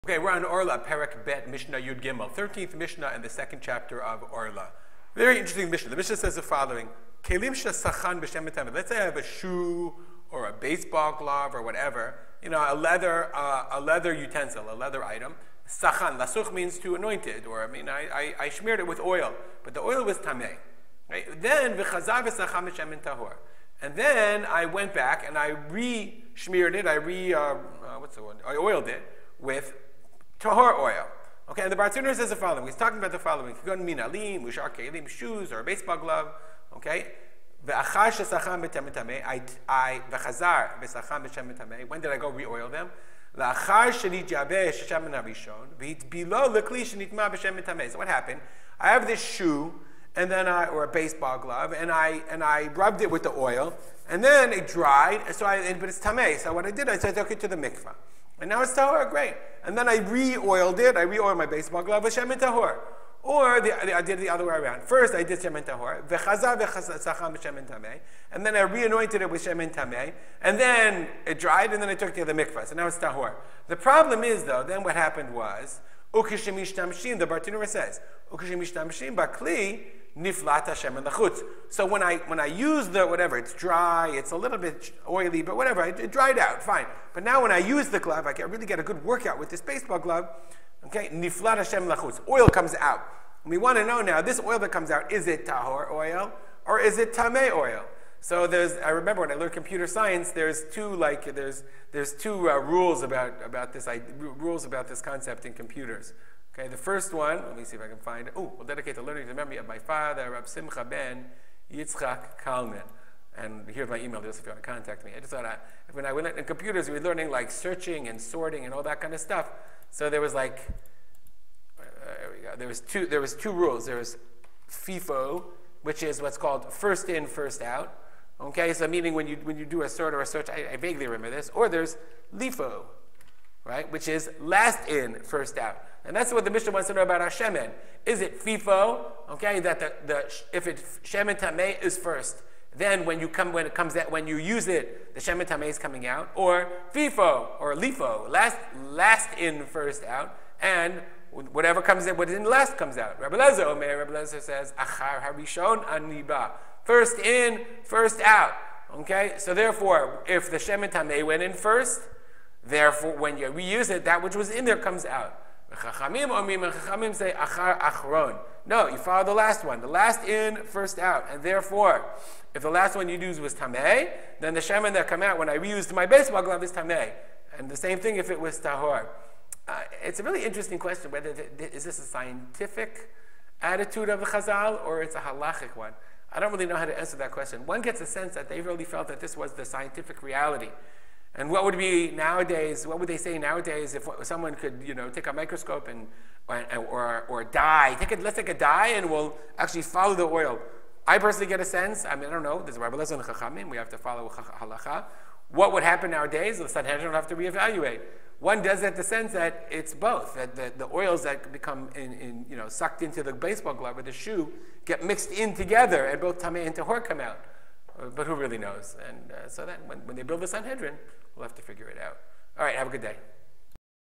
Orlah Chapter 2 Mishnah 13
When oil seeps out of a utensil, is it the first or last oil rubbed in?